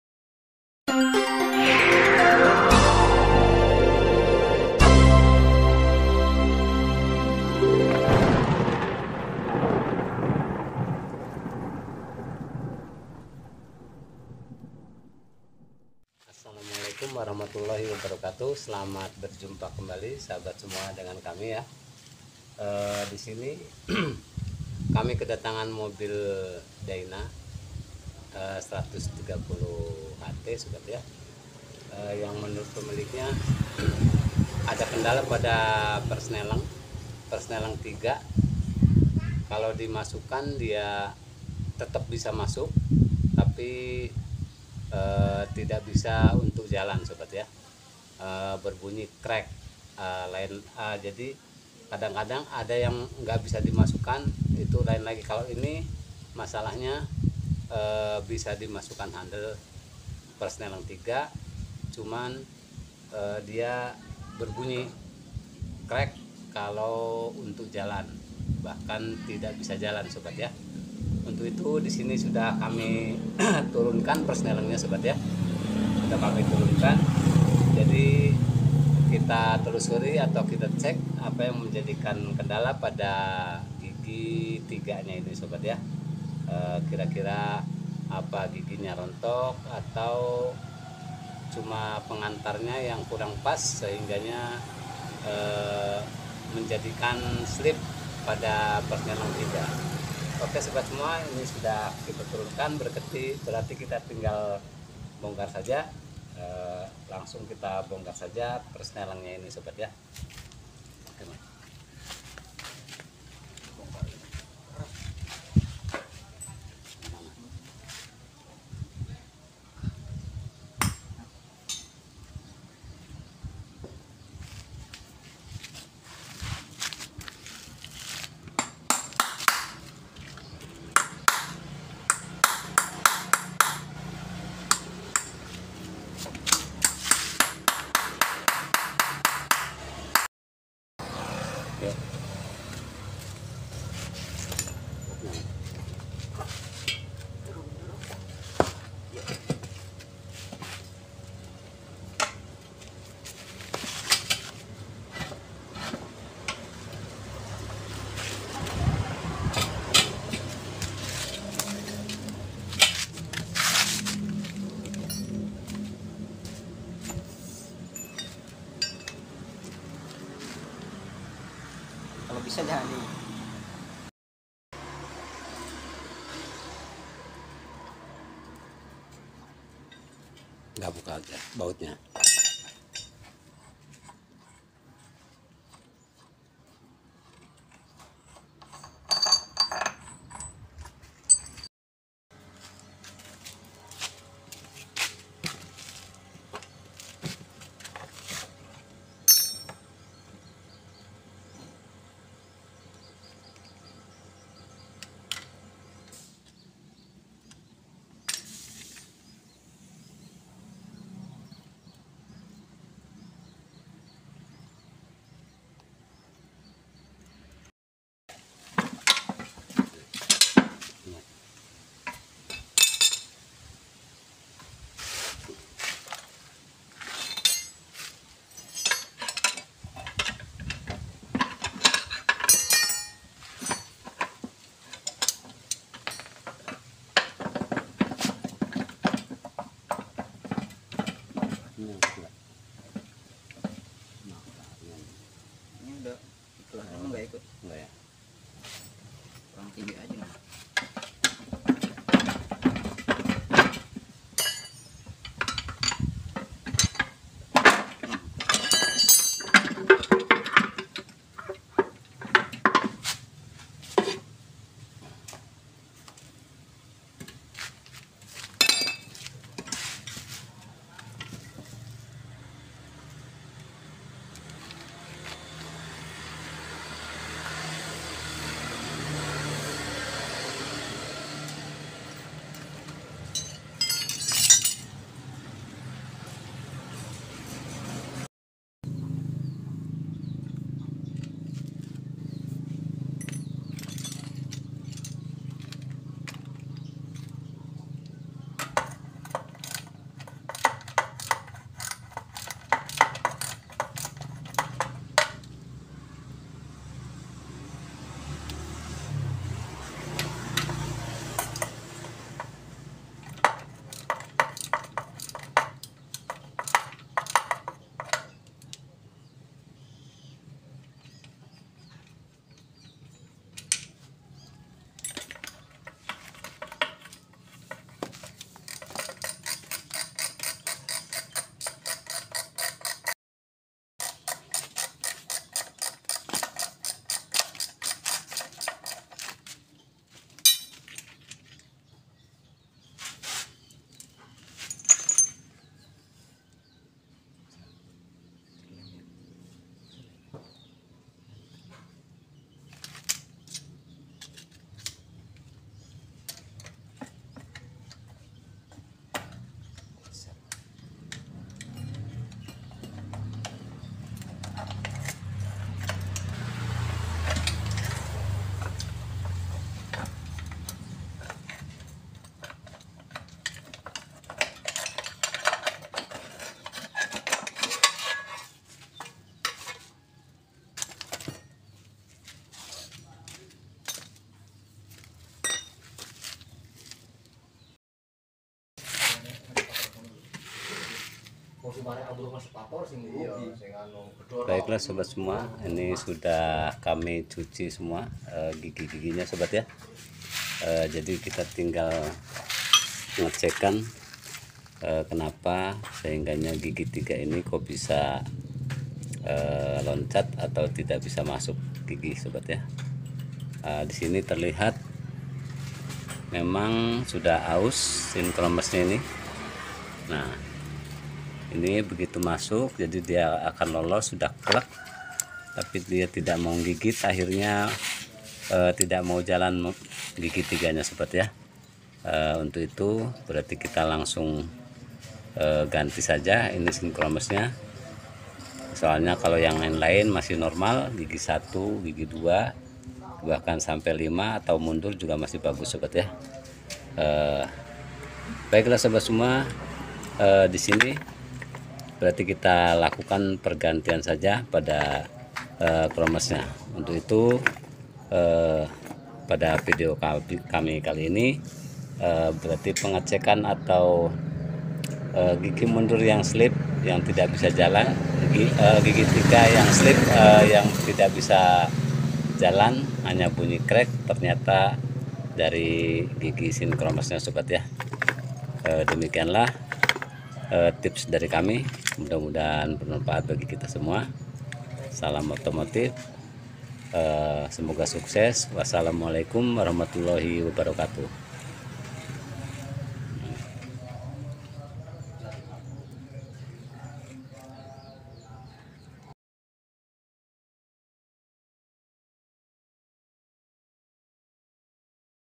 Gigi 3 tidak bisa masuk bunyi krek terkadang lompat || toyota dyna,hino dutro
Penyebab gigi 3 tidak bisa masuk dan bunyi krek di sebabkan ring kromes yng sudah aus